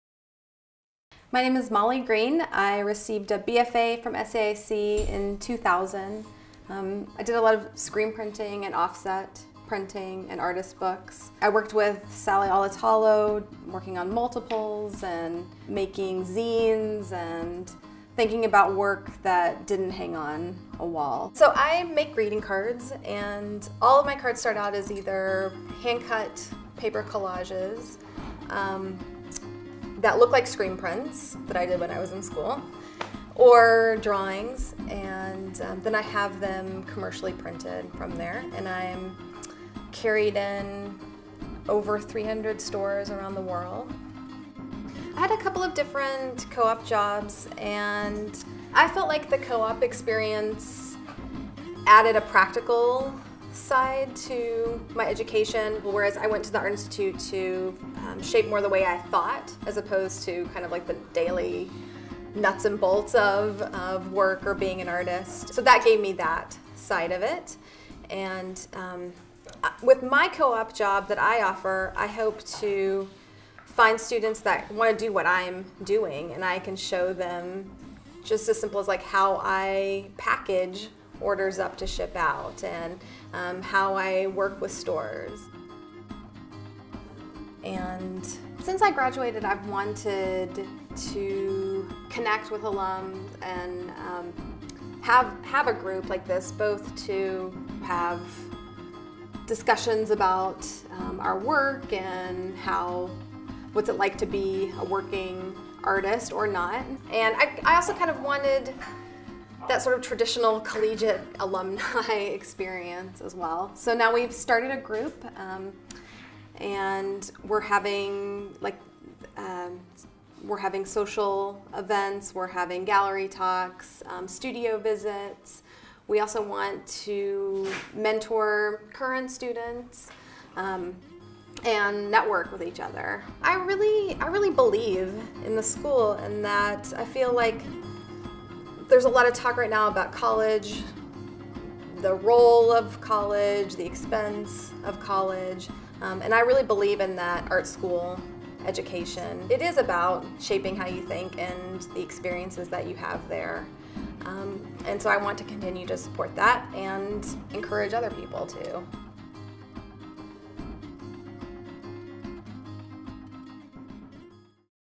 SAIC Alumni Profile: Mollie Green (BFA 2000)
SAIC alum Mollie Green (BFA 2000) combined her retail and artistic skills and founded La Familia Green, a greeting card company based in her Chicago studio. Her cards are printed on recycled paper and sold at independent stores around the world. Her art for the Annual Fund recalls her days as an SAIC student walking from Union Station to the Columbus Drive Building and the infectious energy and creativity she encountered there.